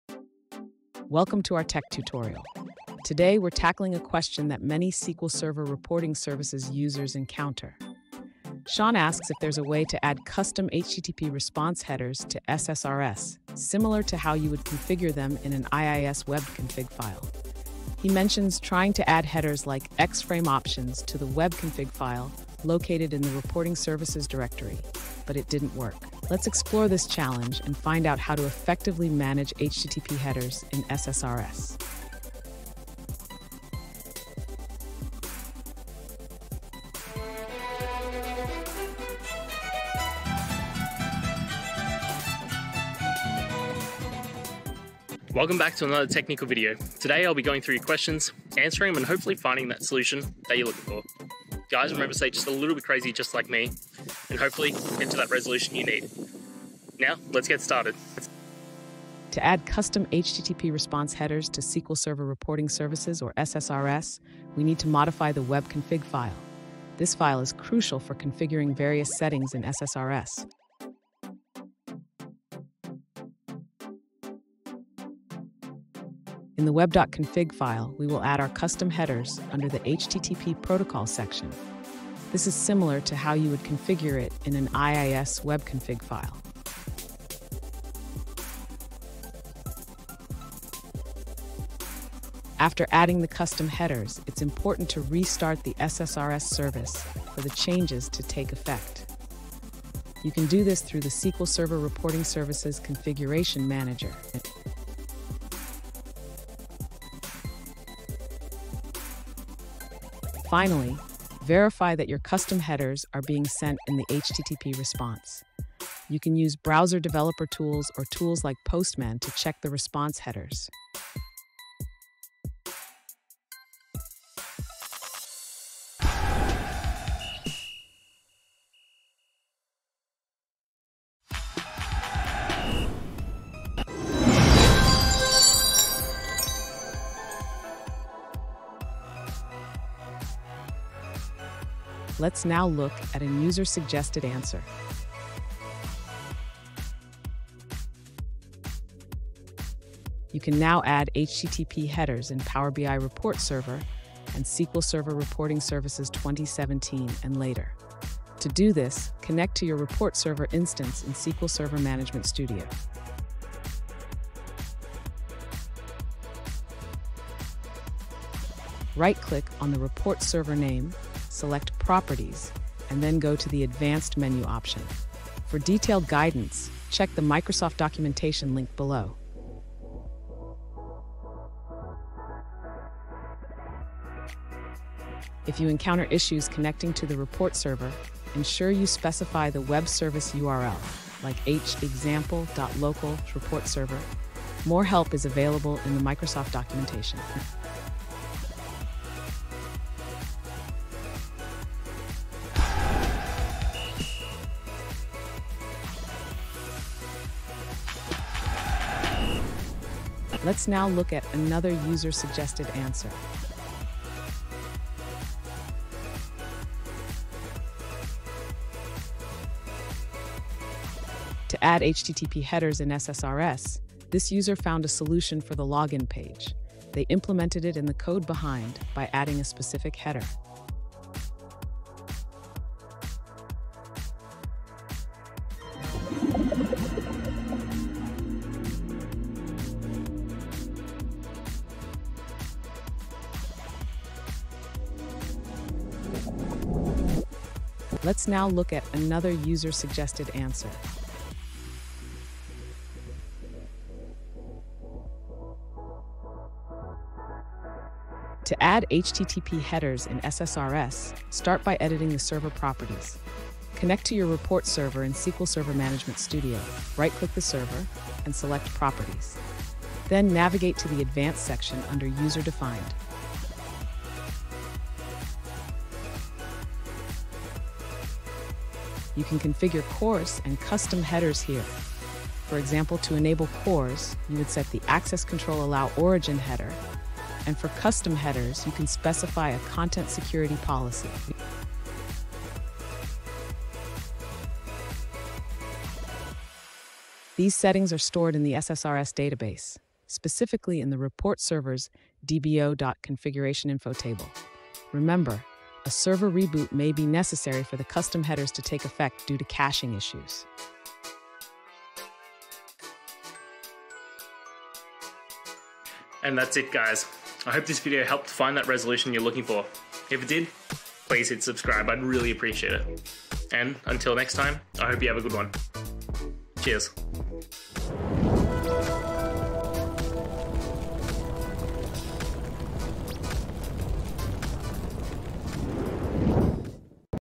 How to Add HTTP Headers to SSRS Reports: A Step-by-Step Guide
In this video, we will explore the essential process of adding HTTP headers to SQL Server Reporting Services (SSRS) reports. Understanding how to manipulate HTTP headers can enhance security, improve performance, and customize the behavior of your reports. Join us as we provide a clear, step-by-step guide to help you implement these changes effectively, ensuring your SSRS reports meet your specific needs.

Today's Topic: How to Add HTTP Headers to SSRS Reports: A Step-by-Step Guide

Related to: httpheaders, ssrsreports, addhttpheaders, step-by-stepguide,